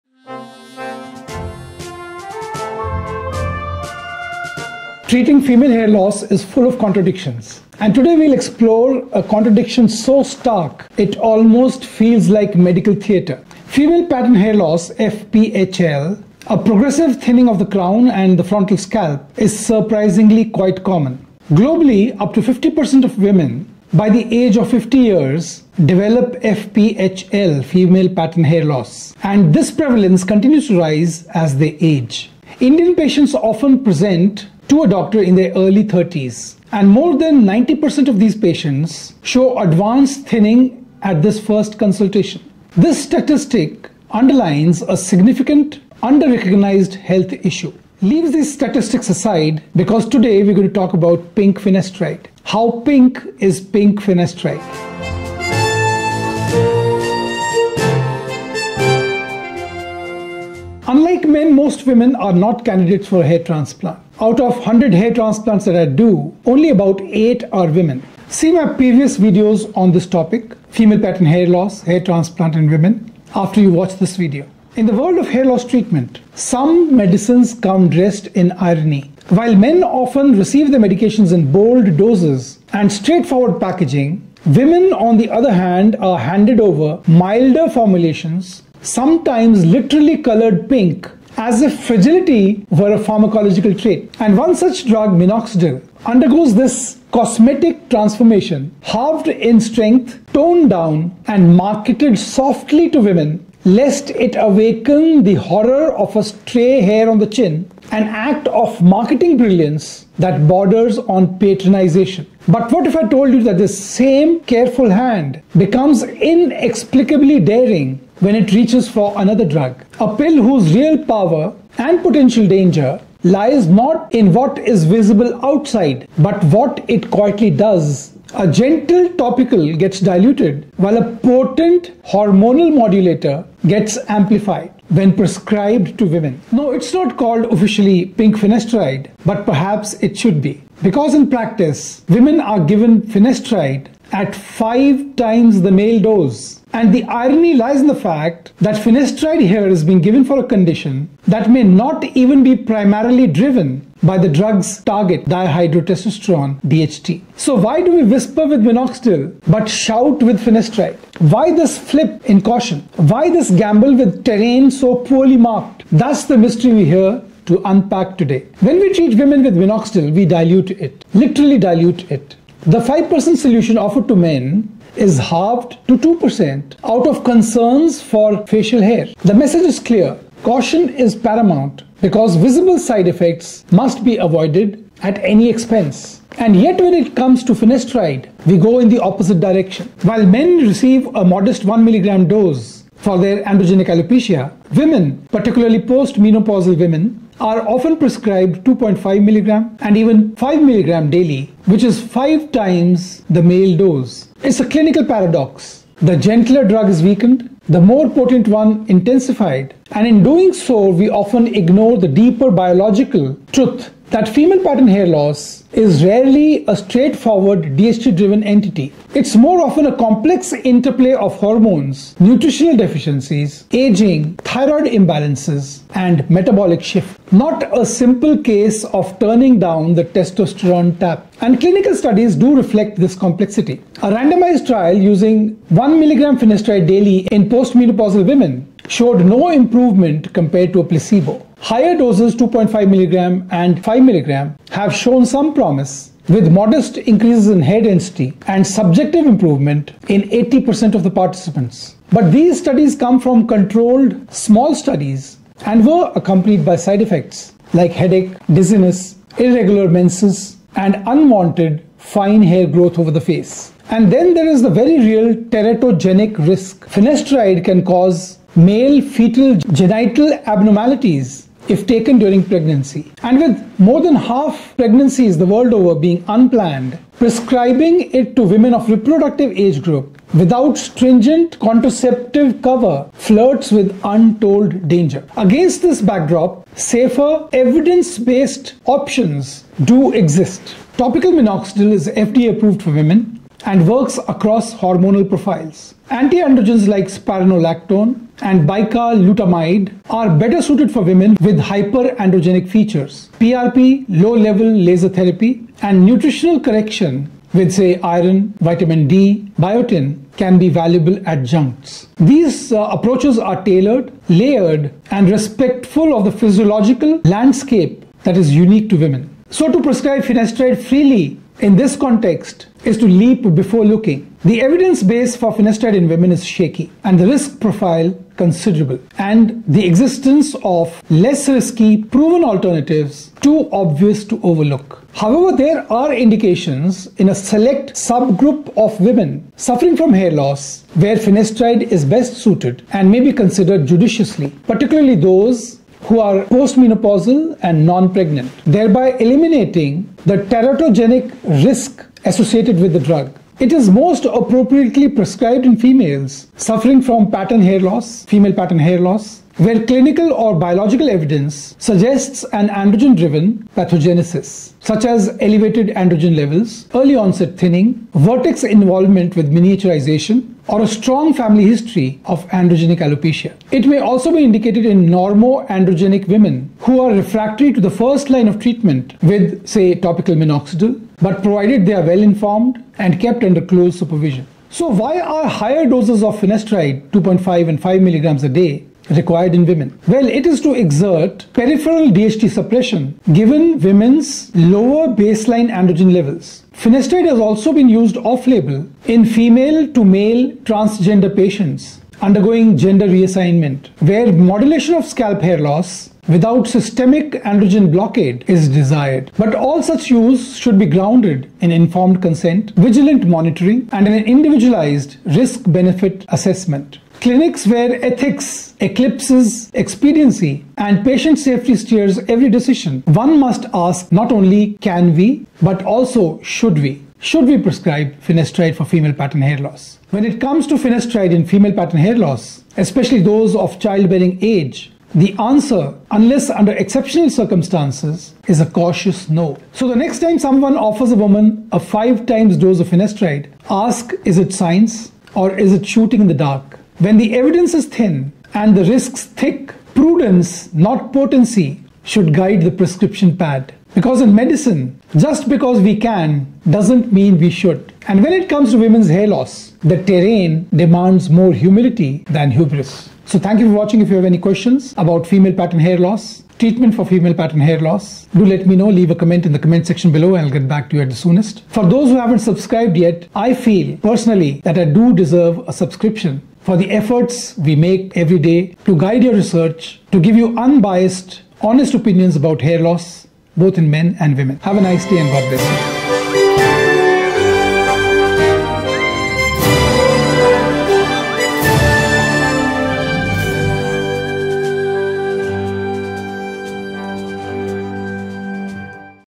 Hair Loss Solution for Female Pattern Hair Loss
“Pink Finasteride?” The Hair‑Loss Paradox in Women You Must Know!

Treating female pattern hair loss (FPHL)—a progressive thinning over the crown and frontal scalp—is more common than many realise: nearly 50 % of women worldwide by age 50, and in India, most women present in their 30s with advanced thinning more than 90 % by their first consult. The paradox? A drug considered gentle—minoxidil—is diluted and “pink‑branded” for women, while finasteride, a potent hormonal modulator, is sometimes given to women at 2.5 mg to 5 mg daily—up to five times the male dose. That’s not marketing nuance; it’s medical irony.

In this video, we unpack:
 Why the 2 % minoxidil solution is standard while finasteride doses escalate.

The shaky evidence: 1 mg/day offers no benefit in postmenopausal FPHL, whereas higher doses show modest gains in small, uncontrolled cohorts (70‑80 % subjective improvement—but with side effects like headaches, dizziness, fine hair growth, menstrual changes)

The real‑life cautionary tale: EMA and FDA now recognise psychiatric and reproductive side effects—including suicidal thoughts, even in lower 1 mg doses—emphasising teratogenic risk in childbearing women

Safer, evidence-based alternatives: topical minoxidil (FDA‑approved), spironolactone, bicalutamide, nutritional correction, PRP, laser therapy.
Watch now to understand:

The clinical paradox of weaker topicals and stronger pills.

Why women often receive five times the finasteride dose of men.

How to approach women’s hair loss with safer, layered protocols grounded in science.

Can women use Men's Rogaine?

00:14 How common is hair loss in women?
00:48 Pink Finasteride for Female Pattern Hair Loss FPHL
01:00 Hair transplant for Female Pattern Hair Loss
01:14 Hair loss treatment for women
03:23 The Paradox of treating FPHL
03:30 Causes of Female pattern hair loss
04:15 Side effects of treatment for female alopecia
04:44 Other treatments for hair loss in women
05:34 Indications for Finasteride in Female Pattern Hair Loss
06:24 Why women need high Finasteride dose than men?